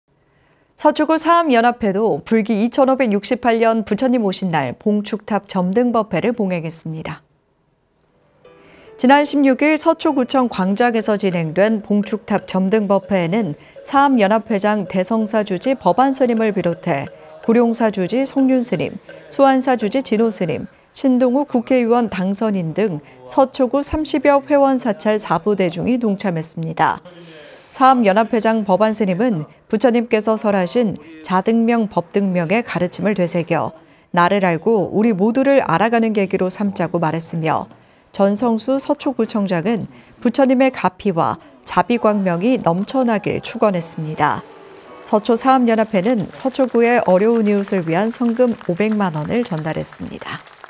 [BTN뉴스] 서초구사암연합회 부처님오신날 봉축탑 점등법회
서초구 사암연합회가 불기 2568년 부처님오신날 봉축탑 점등법회를 봉행했습니다.

지난 16일, 서초구청 광장에서 진행된 봉축탑 점등법회에는 사암연합회장 대성사 주지 법안스님을 비롯해, 구룡사 주지 성륜스님, 수안사 주지 진호스님, 신동욱 국회의원 당선인 등 서초구 30여 회원사찰 사부대중이 동참했습니다.

사암연합회장 법안스님은 “부처님께서 설하신 ‘자등명 법등명’의 가르침을 되새겨 나를 알고, 우리 모두를 알아가는 계기로 삼자”고 말했으며 전성수 서초구청장은 부처님의 가피와 자비광명이 넘쳐나기를 축원했습니다.

서초사암연합회는 서초구에 어려운 이웃을 위한 성금 500만원을 전달했습니다.